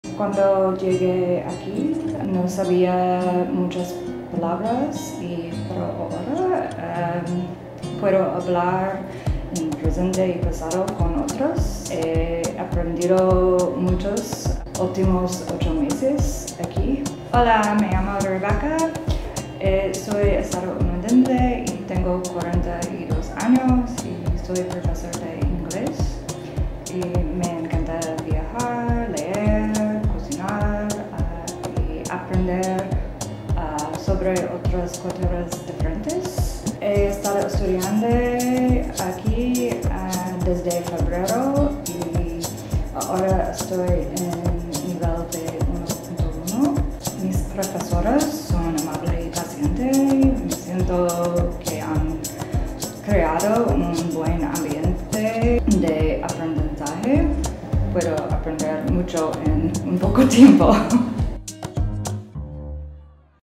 Rebekah | Alumna del mes
La alumna del mes es Rebekah Morgan. Ella es de Estados Unidos, es profesora, lleva ocho meses estudiando en Bcnlip ¡y ya está en el nivel B1.1! Aquí podéis conocer un poco más sobre ella.

The student of the month is Rebekah Morgan. She is from the United States, she is a teacher and has been studying at Bcnlip for eight months - and she is already at B1.1 level! Here you can learn a bit more about her.